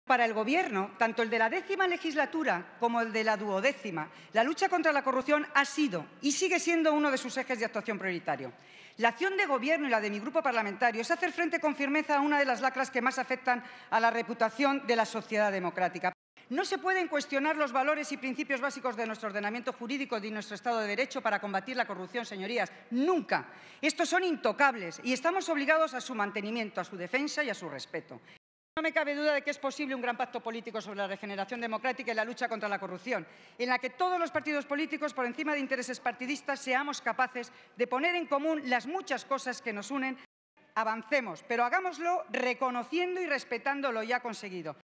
"La acción del Gobierno y del GPP se basa en hacer frente a la lacra de la corrupción"
La portavoz en la Comisión de Calidad Democrática del GPP, Beatriz Escudero, Beatriz Escudero, interviene ante el Pleno del Congreso durante el debate de la Proposición de Ley Integral de lucha contra la corrupción y protección de los denunciantes